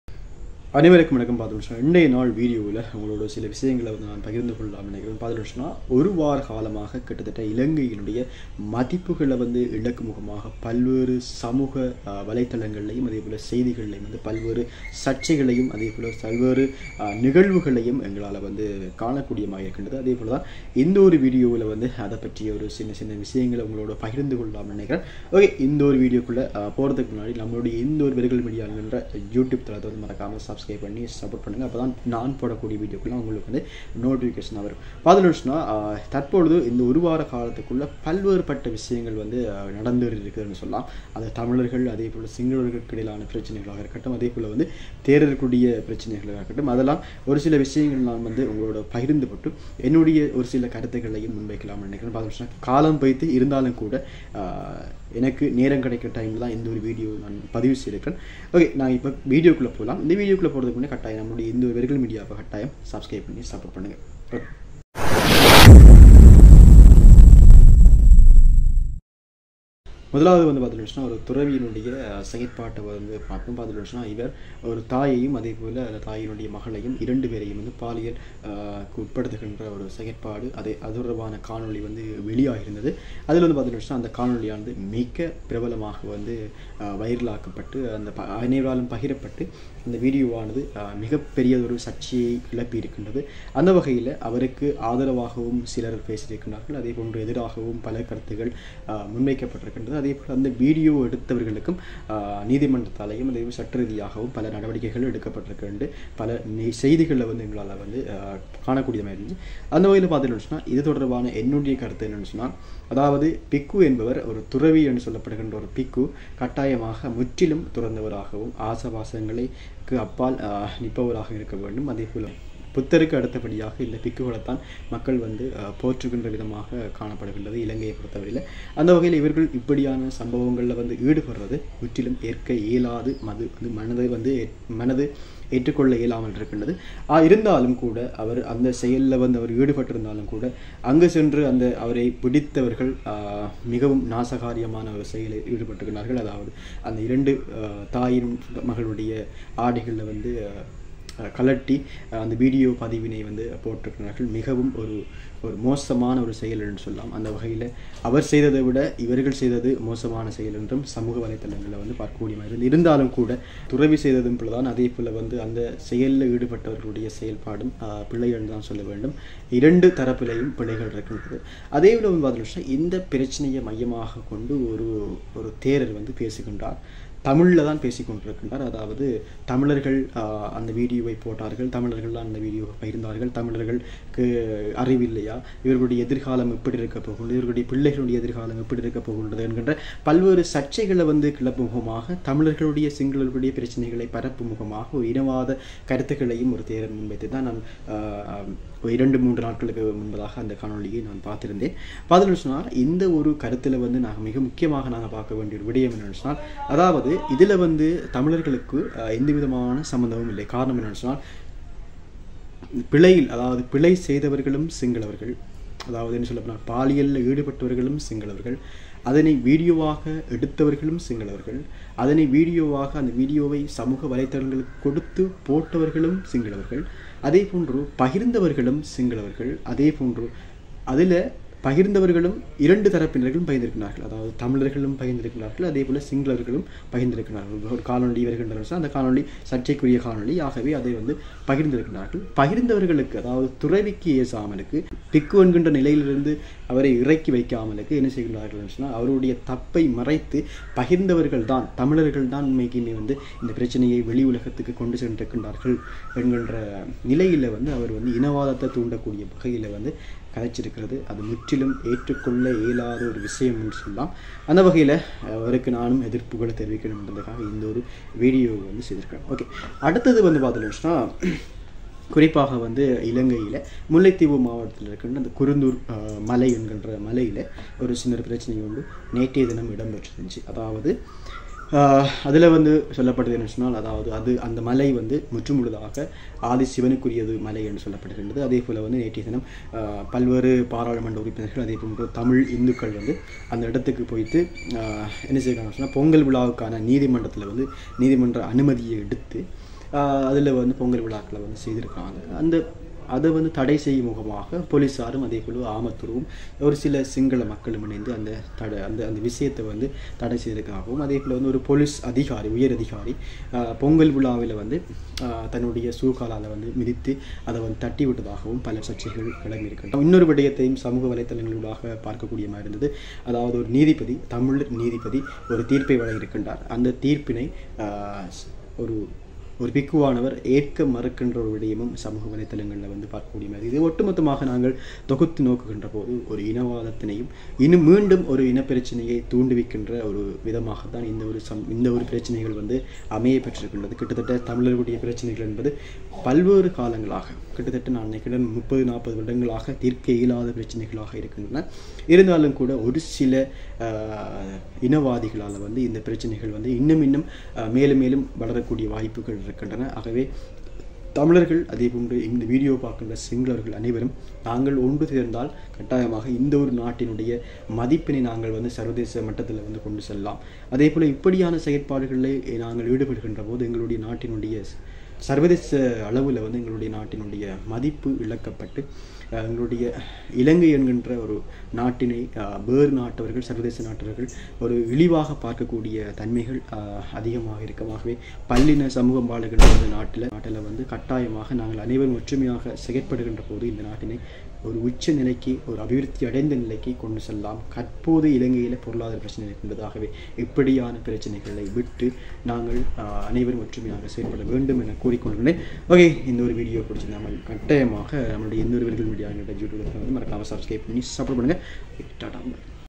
தாய் மகளுடன் உடலுறவில் ஈடுபட்ட பிக்கு 😱srilanka
இலங்கையில் ஒரு வார காலத்திற்குள் பல்வேறு சமூகப் பிரச்சனைகளை ஏற்படுத்தும் முகமாக நிகழ்வுகள் இடம்பெற்றுள்ளது..
தாய் மகளுடன் பிடிபட்ட பிக்கு
குருந்தூர்மலை